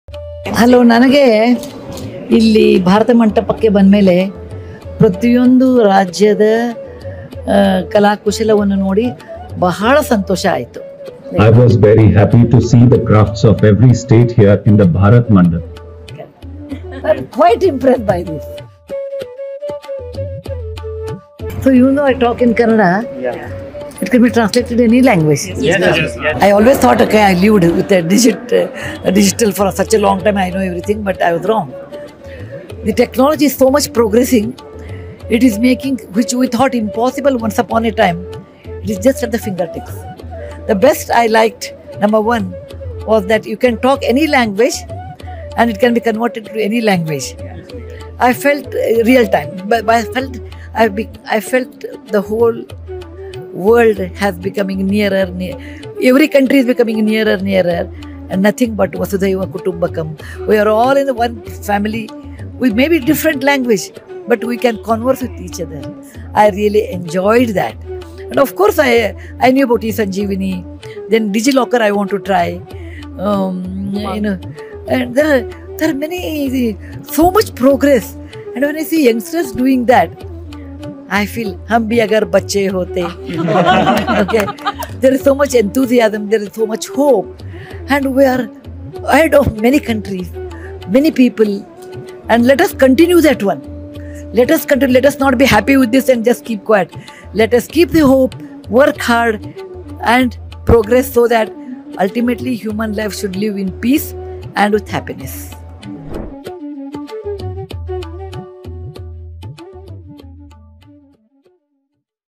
Said Rajya Sabha MP and philanthropist Sudha Murty after exploring the application
🔉Listen 🔉

DigitalIndia initiatives like Bhashini are bringing the world closer, said Rajya Sabha MP and philanthropist Sudha Murty after exploring the application at the Digital India Experience Zone @

She appreciated the young innovators for making such useful solutions and motivated them to keep up the good work!